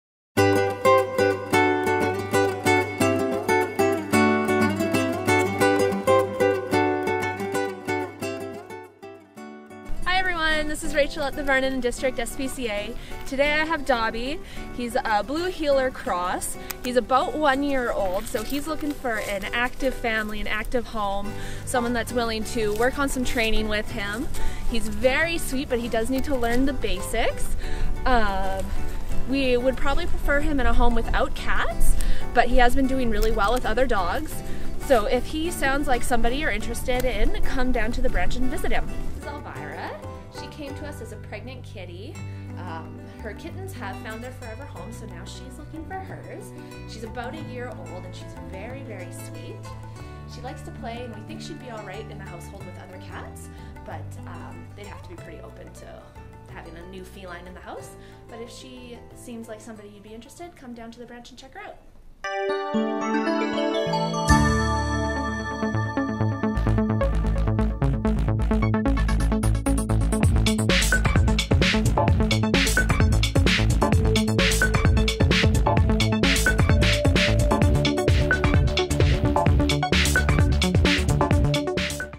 Doby and Elvira
Doby and Elvira are ready for adoption